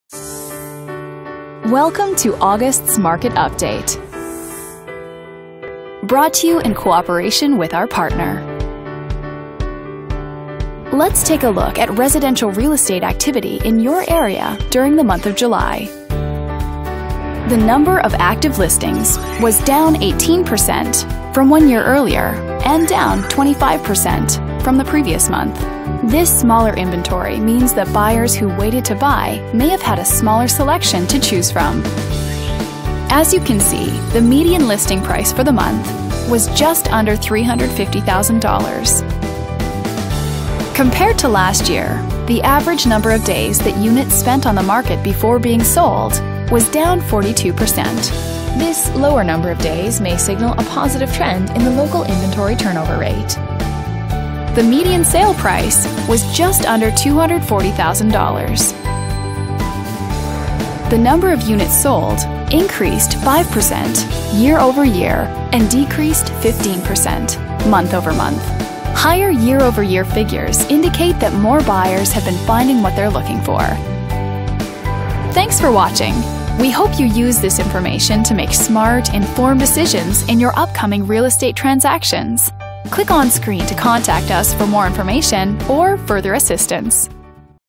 East Nashville,TN, Real Estate Market Update from PARKS,August, 2018
PARKS Video Update recapping market activity within East Nashville, TN, during the month of August, 2018